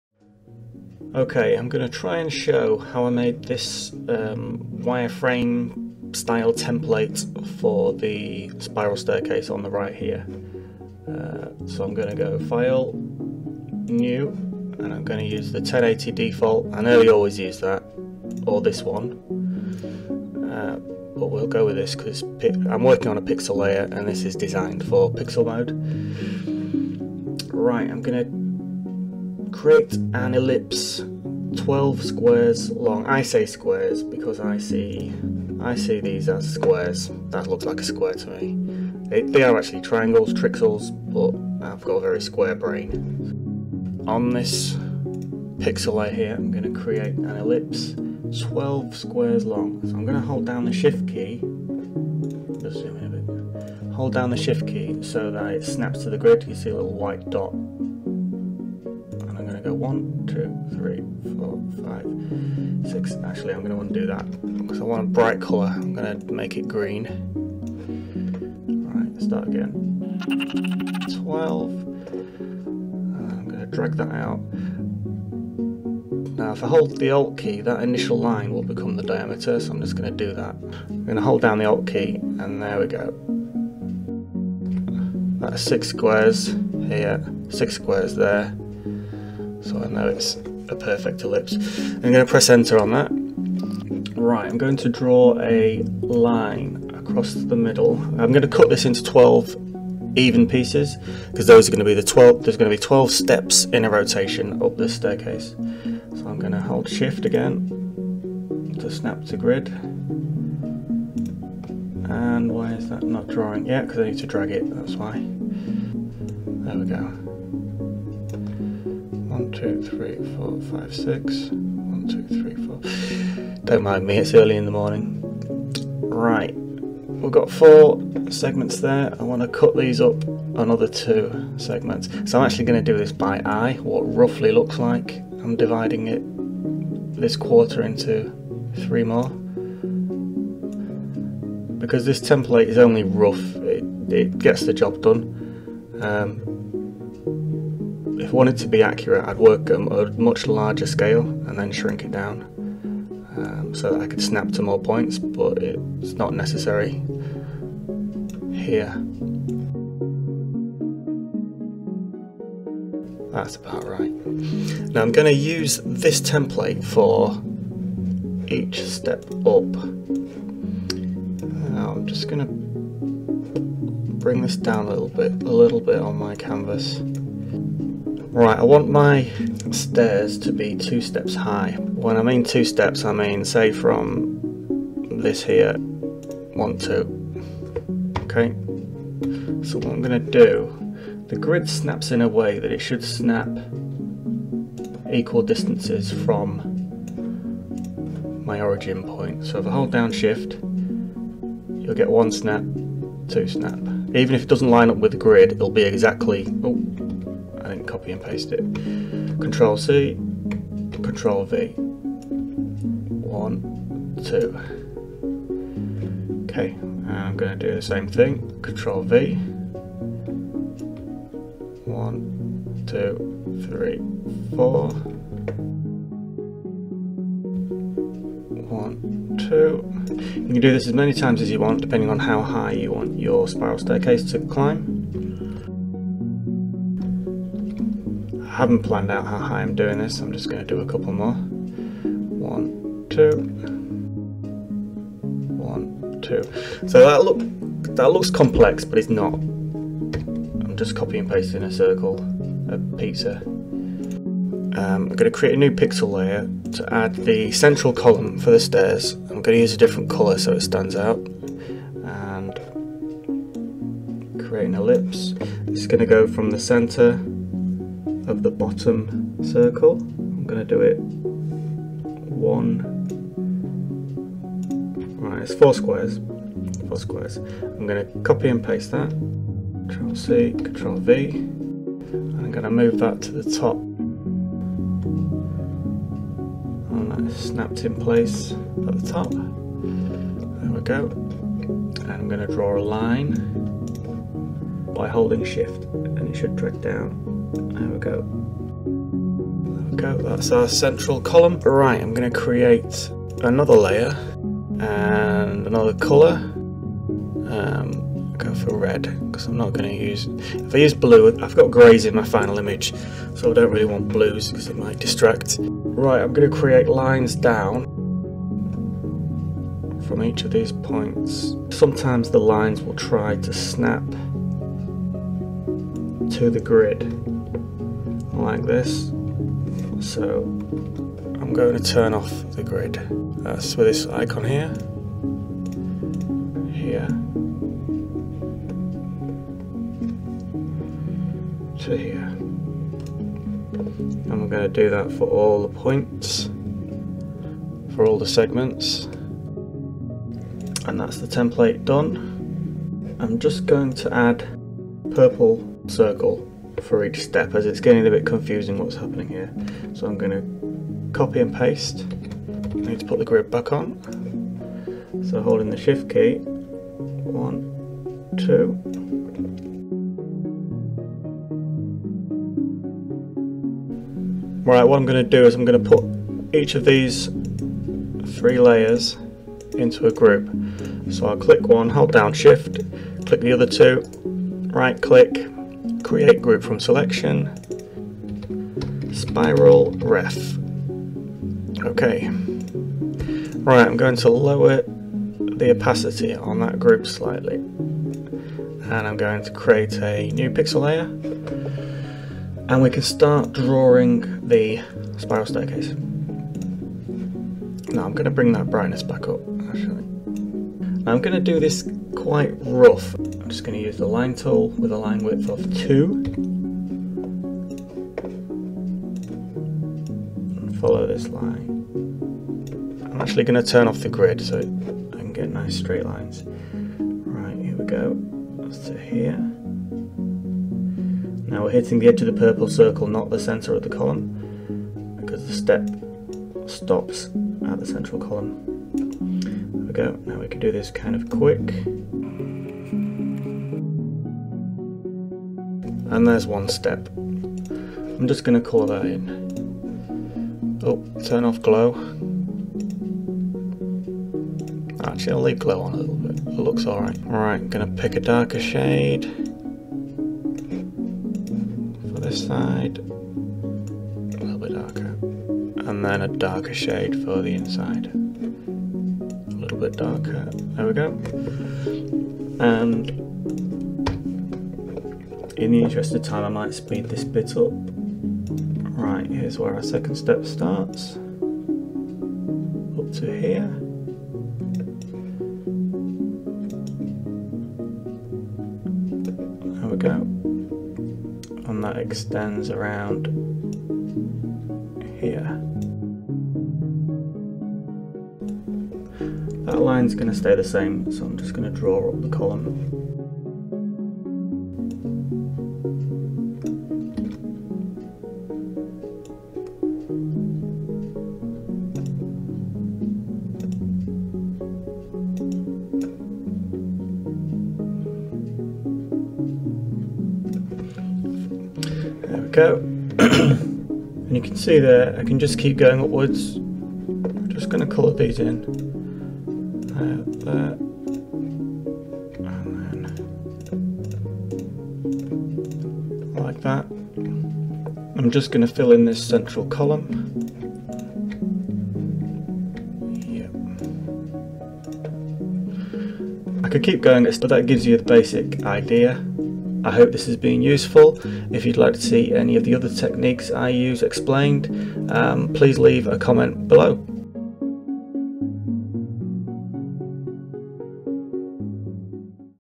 HEXELS Spiral Stairs Tutorial
a quick video explaining how I created a reference template for an isometric staircase. There may be a better way to do it but this is just my technique. I'm trying something new with a commentary track. Something i'm not used to so please be gentle.